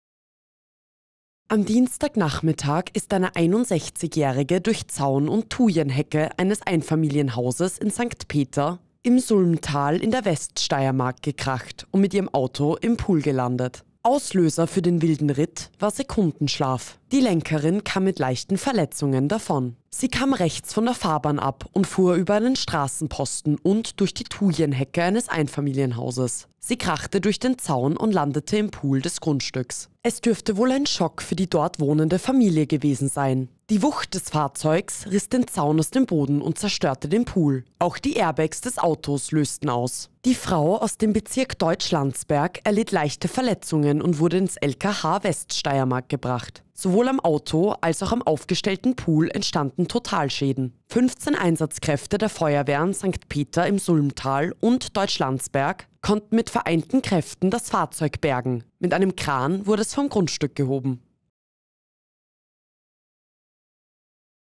Sekundenschlaf: Steirerin landete mit Auto in Pool | krone.tv NEWS
Am Dienstagnachmittag ist eine 61-Jährige durch Zaun und Thujenhecke eines Einfamilienhauses in St. Peter im Sulmtal in der Weststeiermark gekracht und mit ihrem Auto im Pool gelandet. Auslöser für den wilden Ritt war Sekundenschlaf. Die Lenkerin kam mit leichten Verletzungen davon.